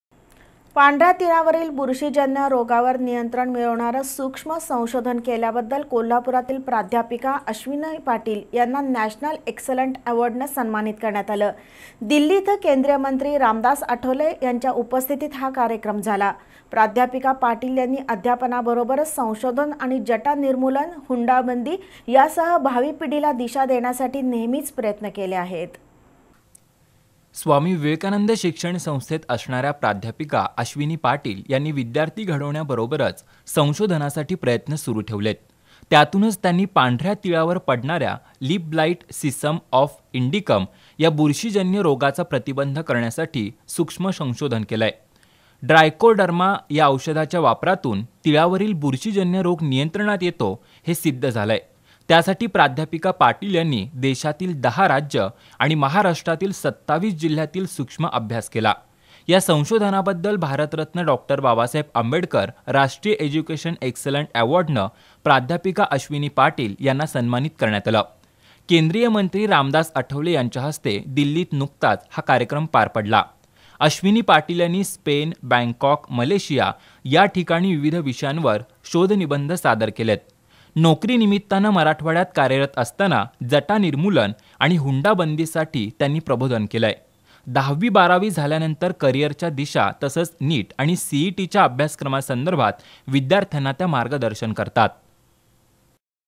कोल्हापुरातील प्राध्यापिका अश्‍विनी पाटील नॅशनल एक्सलंट ऍवॉर्डनं सन्मानीत
पांढर्‍या तिळावरील कीड रोखण्यासाठीच्या संशोधनाबद्दल कोल्हापुरातील प्राध्यापिका अश्‍विनी पाटील नॅशनल एक्सलंट ऍवॉर्डनं सन्मानीत
Channel B